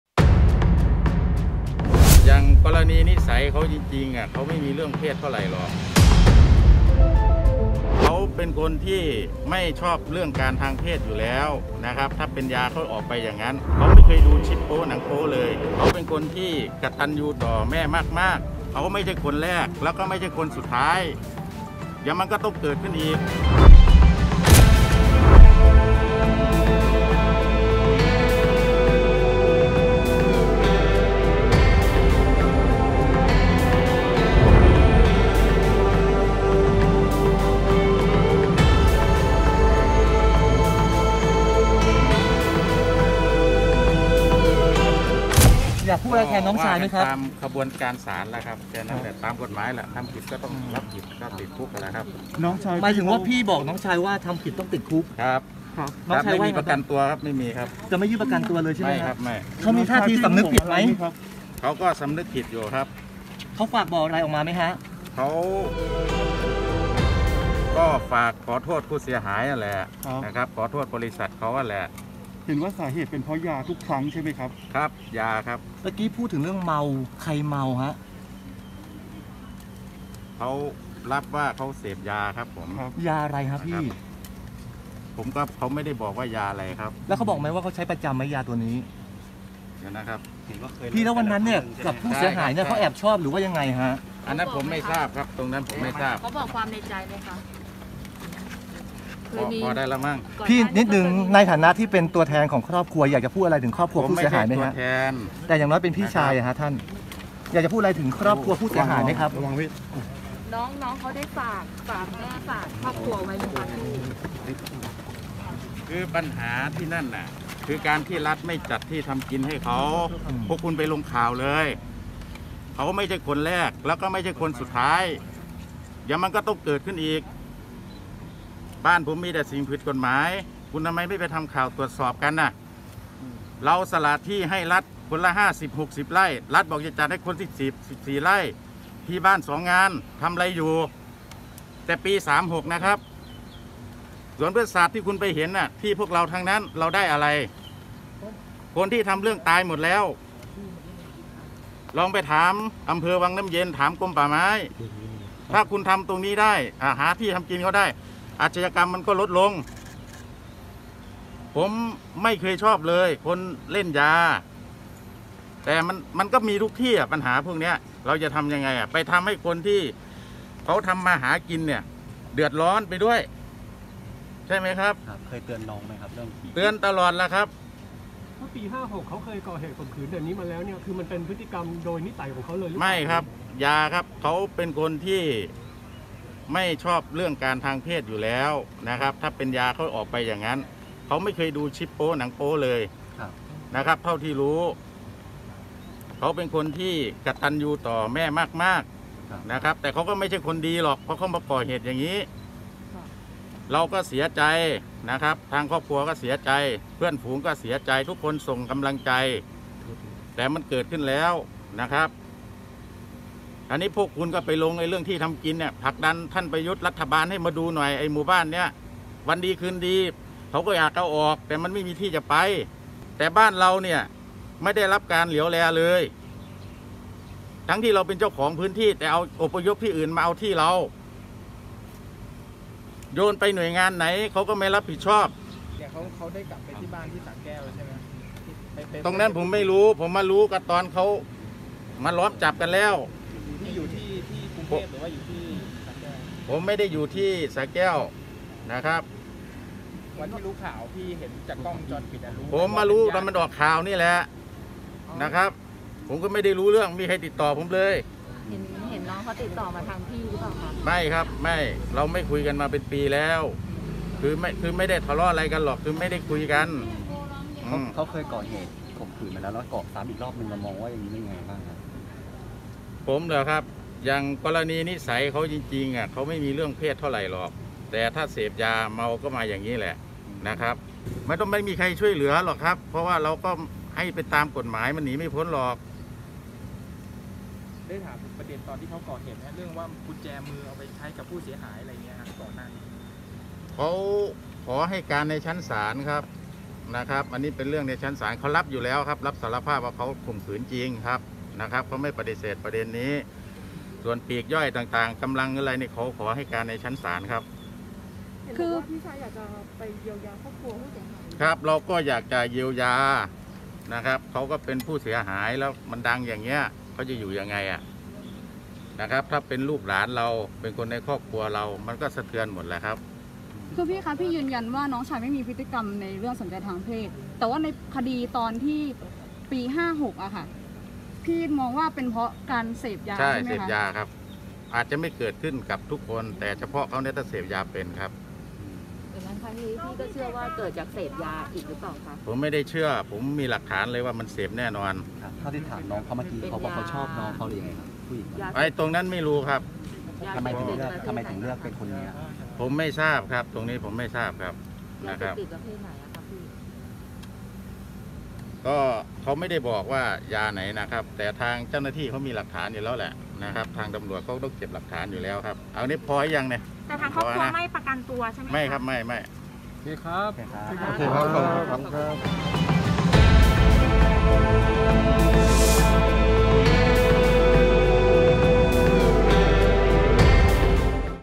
พี่ รปภ. ลั่น น้องเป็นคนกตัญญู ไม่เคยดูคลิปโป๊ น้องไม่ใช่คนแรก และไม่ใช่รายสุดท้าย!! : Khaosod TV
พี่หัวหน้า รปภ. ลั่น น้องเป็นคนกตัญญู ไม่เคยดูคลิปโป๊ น้องไม่ใช่คนแรก และไม่ใช่รายสุดท้าย!!